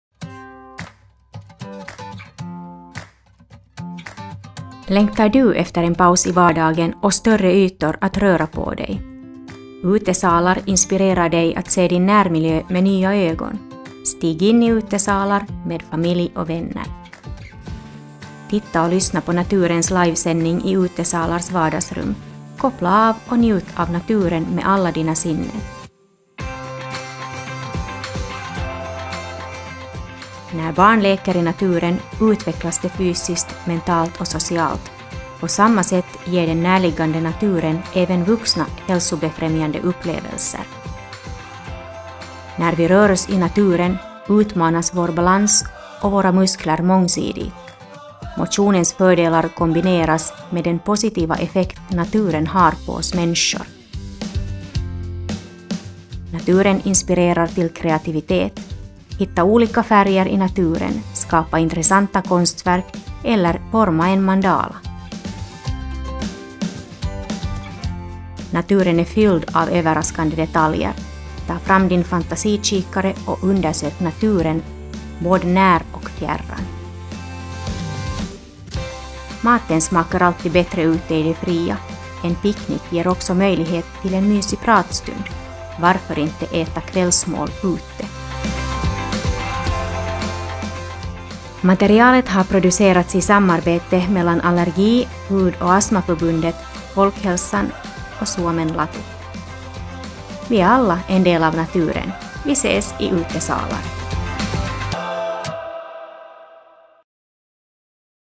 Utesalar
Presentation av alla rummen i Utesalar materialet. Vardagsrum, träningssal, ateljé, laboratorium, lekplats och matplats